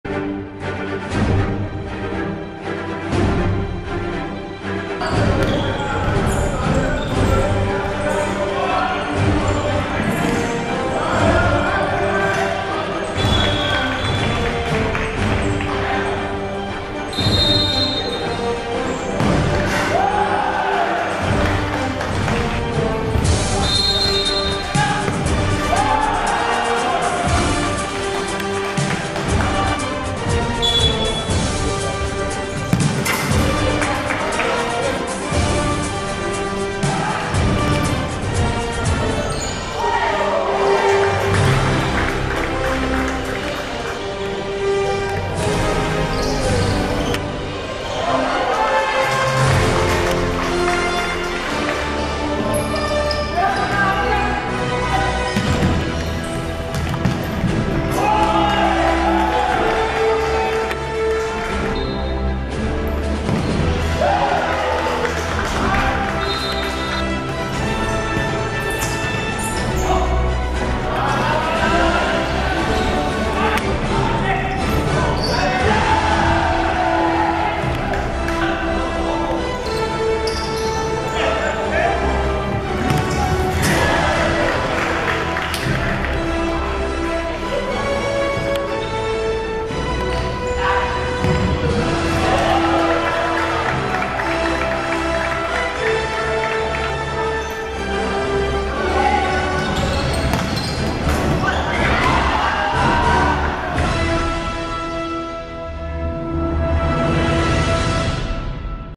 車路士五人足球聯賽第四週(2015.2.16.)入球精輯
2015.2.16.石硤尾體育館 車路士五人足球聯賽第四週 是日共有12個入球